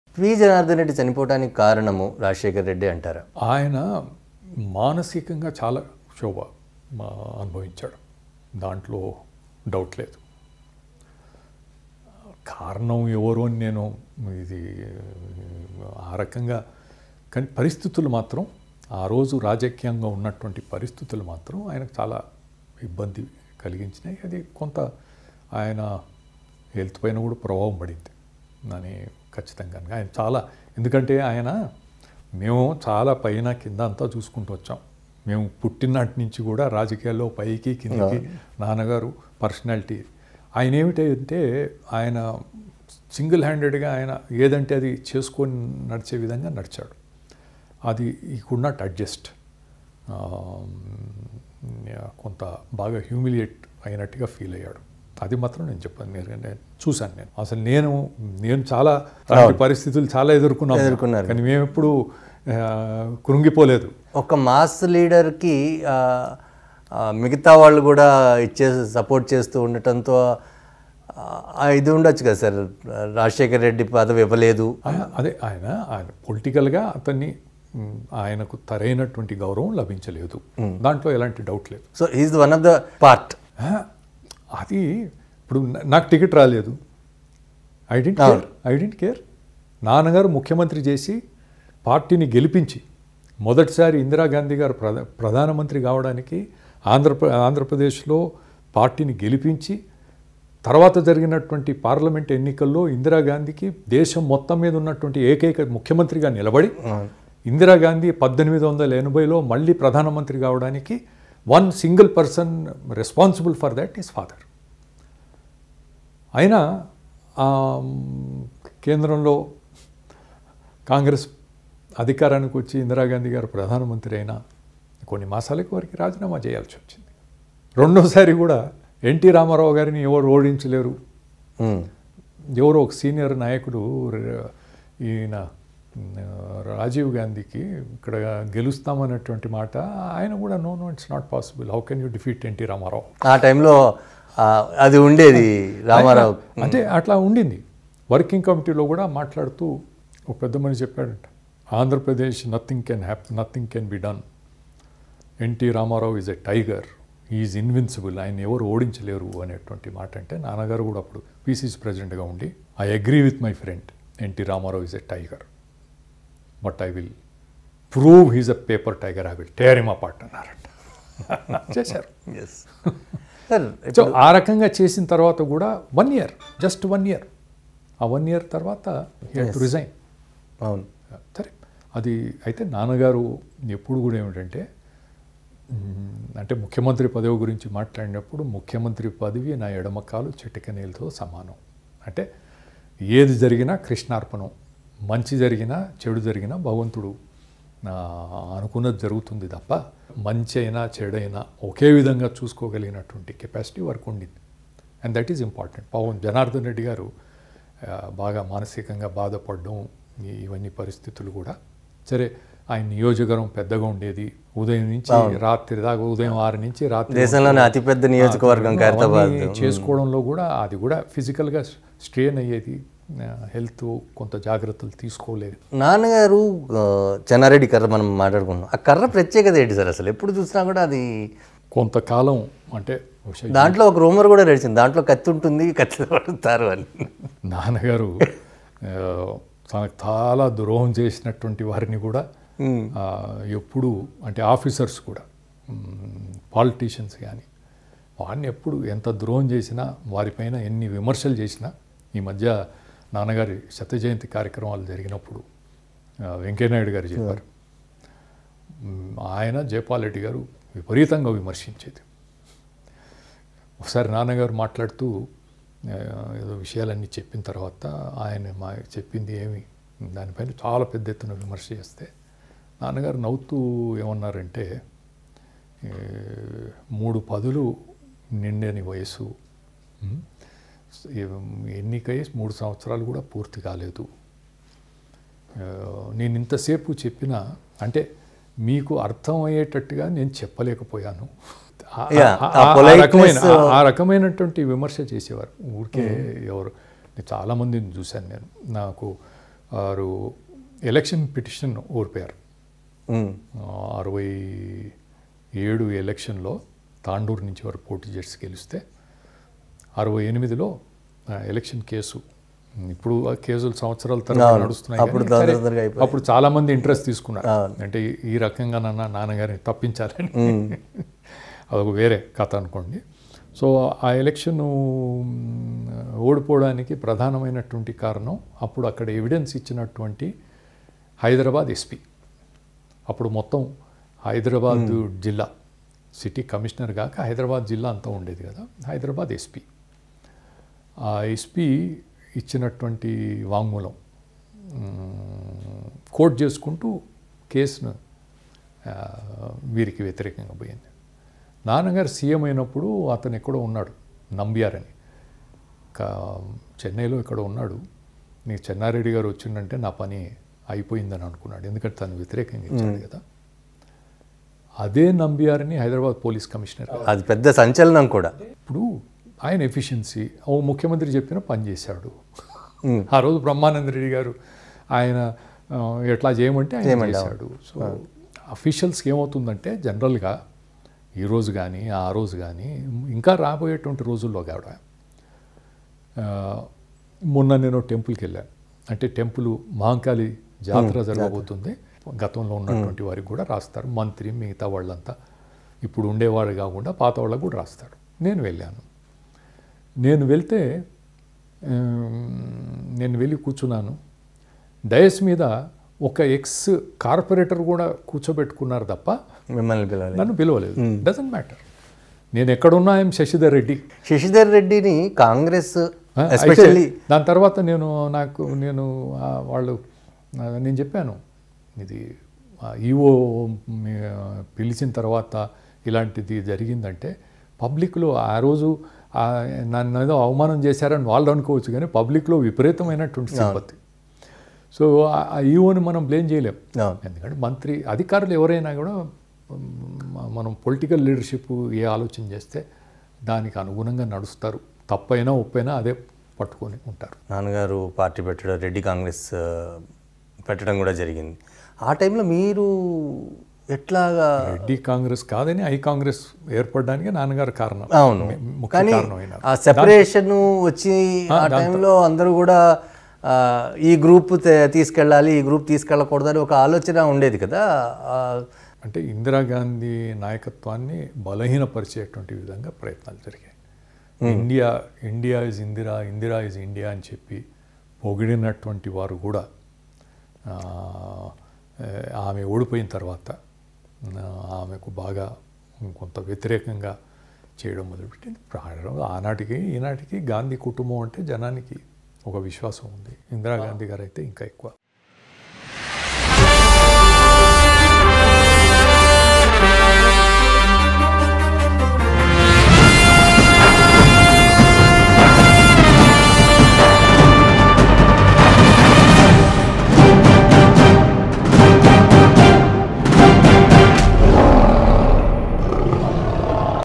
పి జనార్దన్ రెడ్డి మృతికి కారణం రాజశేఖర్ రెడ్డి..? | Marri Shashidhar Reddy about P Janardhan Reddy
Leo Politics

Leo Health

Leo Entertainment

Leo Devotional